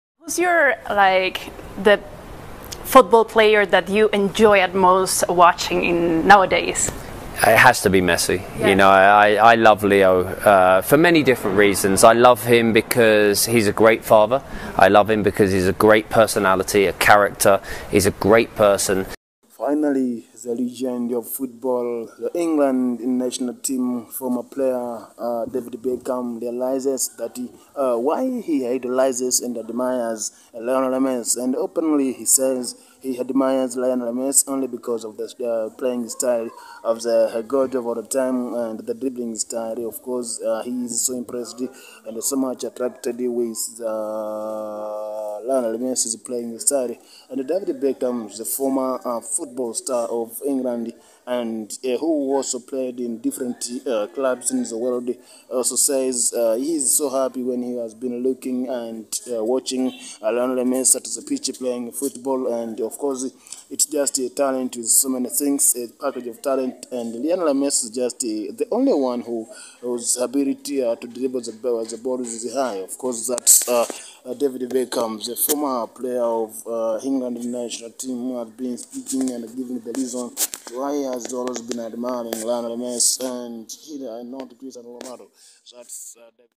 David Beckham " I enjoy & love watching Messi on the pitch, one of the few entertainer of this game"
David Beckham reveals why he idolizes and admires Messi